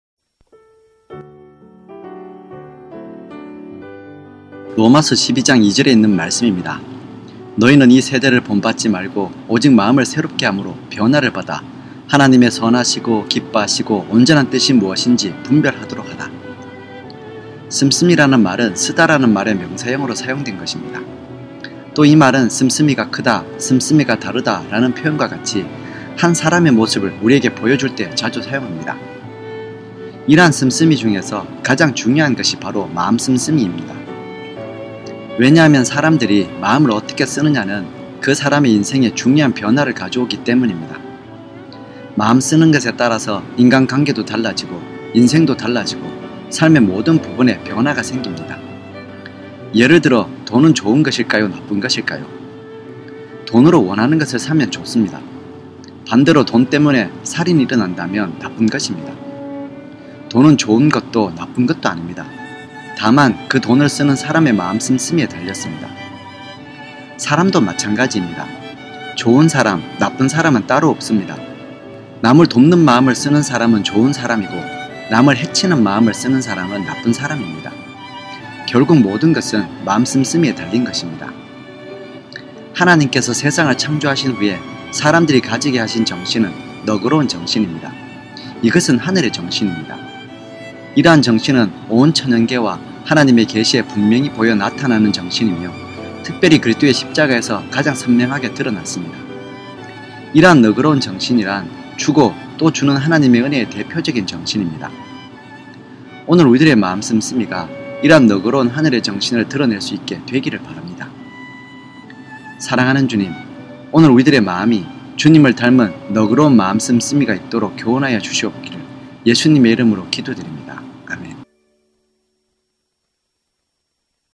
[새벽성서학교 2분설교] 마음 씀씀이
2020년 11월 2일(월) 마음 씀씀이
“너희는 이 세대를 본받지 말고 오직 마음을 새롭게 함으로 변화를 받아 하나님의 선하시고 기뻐하시고 온전하신 뜻이 무엇인지 분별하도록 하라” (로마서 12:2)
 “씀씀이”라는 말은 “쓰다”라는 말의 명사형으로 사용한 것입니다. 또 이 말은 “씀씀이가 크다, 씀씀이가 다르다”라는 표현과 같이 한 사람의 모습을 우리에게 보여줄 때 자주 사용합니다. 이러한 씀씀이 중에서 가장 중요한 것이 바로 “마음 씀씀이”입니다. 왜냐하면 사람들이 마음을 어떻게 쓰느냐?는 그 사람의 인생에 중요한 변화를 가져오기 때문입니다. 마음 쓰는 것에 따라서 인간관계도 달라지고 인생도 달라지고 삶의 모든 부분에 변화가 생깁니다. 예를 들어 돈은 좋은 것일까요? 나쁜 것일까요? 돈으로 원하는 것을 사면 좋습니다.  반대로 돈 때문에 살인이 일어나면 나쁜 것입니다. 돈은 좋은 것도 나쁜 것도 아닙니다.  다만, 그 돈을 쓰는 사람의 마음 씀씀이에 달렸습니다. 사람도 마찬가지입니다. 좋은 사람 나쁜 사람은 따로 없습니다. 남을 돕는 마음을 쓰는 사람은 좋은 사람이고, 남을 해치는 마음을 쓰는 사람은 나쁜 사람입니다. 결국 모든 것은 마음 씀씀이에 달린 것입니다. 하나님께서 세상을 창조하신 후에 사람들이 가지게 하신 정신은 “너그러운 정신”입니다. 이것은 하늘의 정신입니다. 이러한 정신은 온 천연계와 하나님의 계시에 분명히 보여 나타나는 정신이며 특별히 그리스도의 십자가에서 가장 선명하게 드러났습니다. 이러한 너그러운 정신이란 “주고 또 주는 하나님의 은혜”의 대표적인 정신입니다. 오늘 우리들의 마음 씀씀이가 이런 너그러운 하늘의 정신을 드러낼 수 있게 되기를 바랍니다.
 사랑하는 주님 오늘 우리의 마음이 주님을 닮은 너그러운 마음 씀씀이가 있도록 교훈하여 주시옵기를 예수님의 이름으로 기도드립니다. 아멘